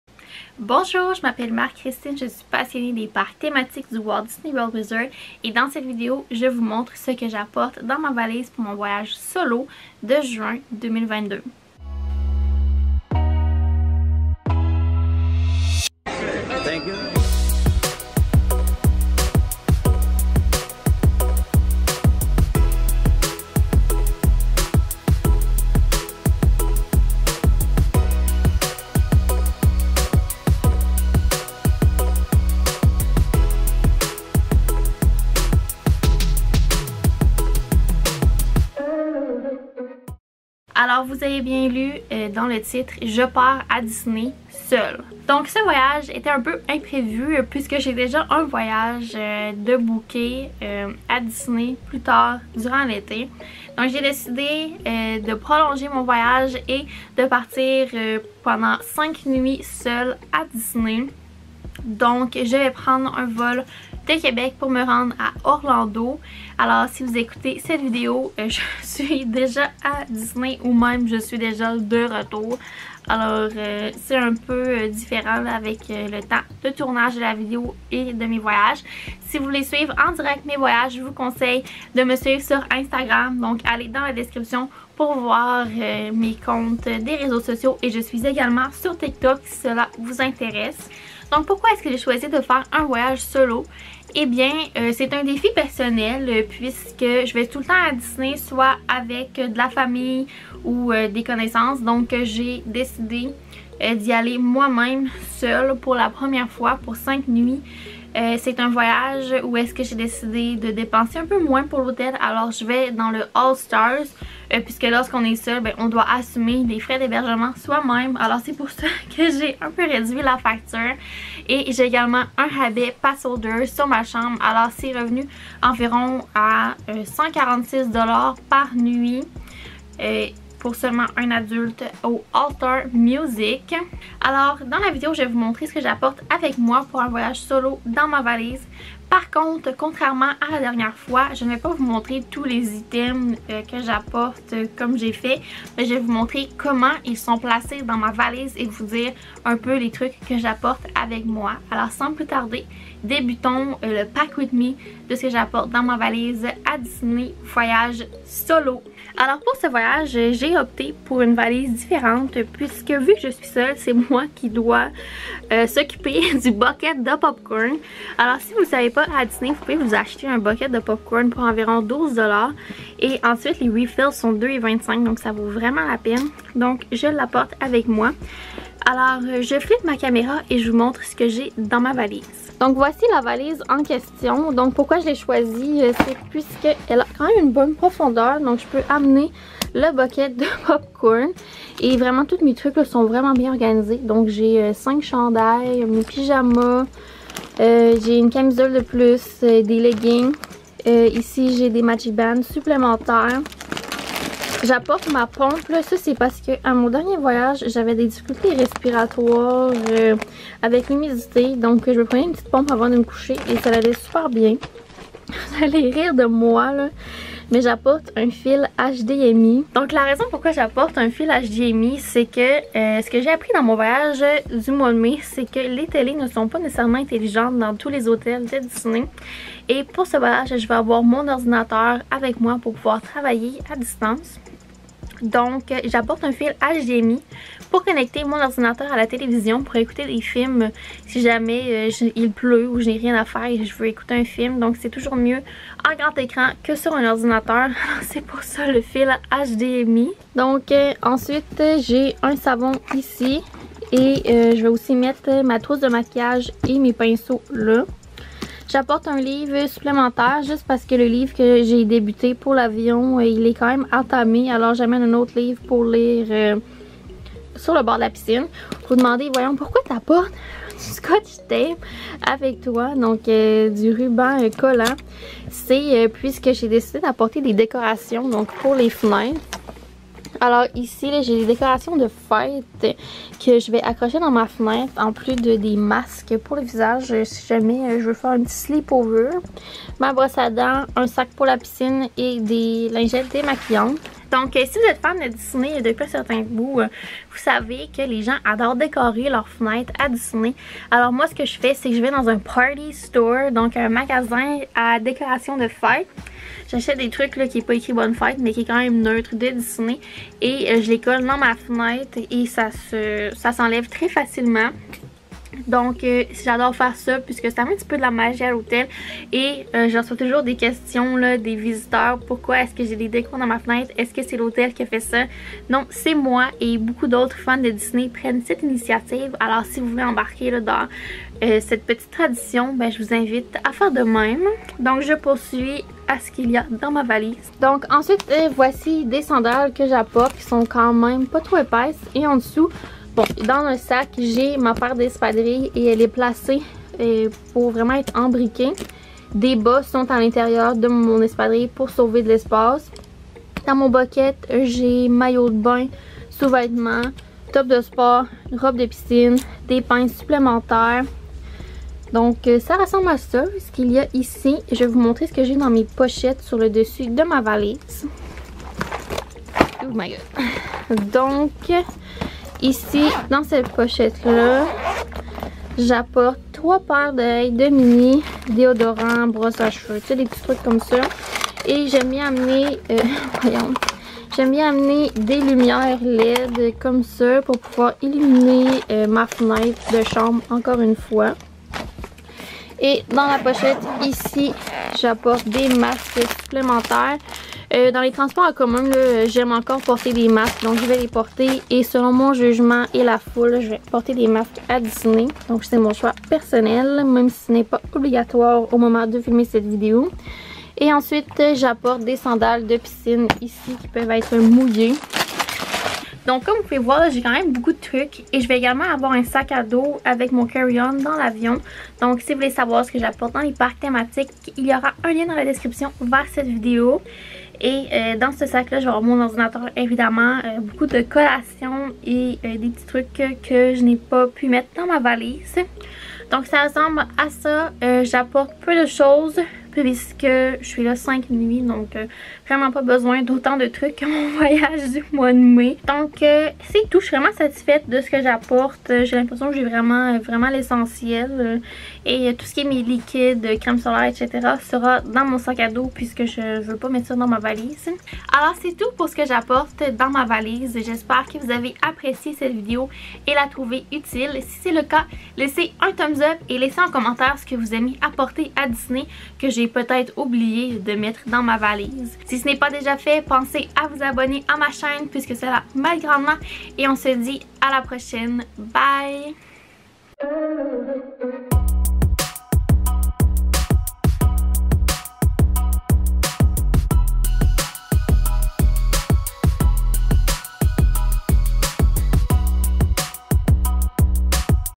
✨ VOYAGE SOLO ✨| Ce que j’apporte à Disney World ✈️| Juin 2022
Dans cette vidéo, je partage ce que j’apporte au Walt Disney World Resort en été.

Instagram & TikTok: @disneyavecmc